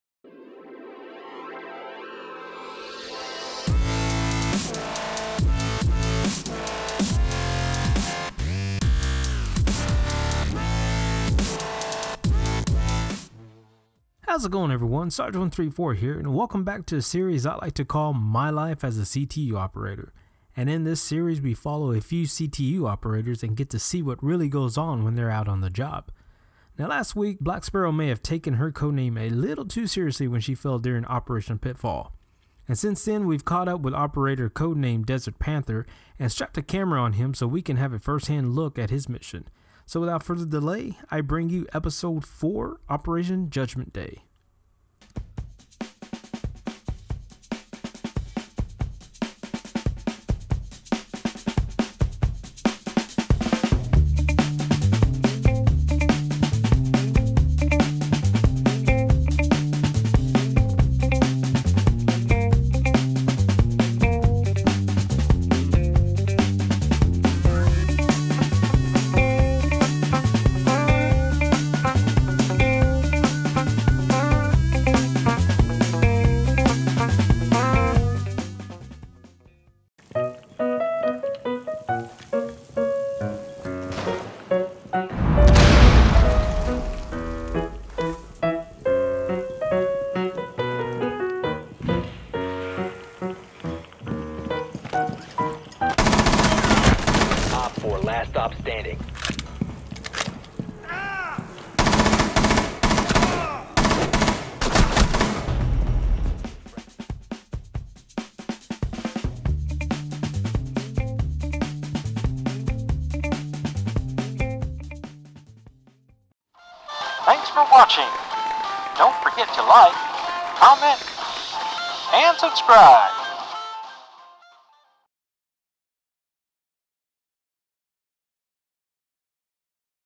My Life as a CTU Operator - Episode 4 "Operation Judgement Day"
Just another day at the office... Well Mr. Bad Guy it was either me or you and well I have a wife and kids to feed...

Welcome to "My life as a CTU Operator" — Day 4

Audience applause recorded by Matthiew11.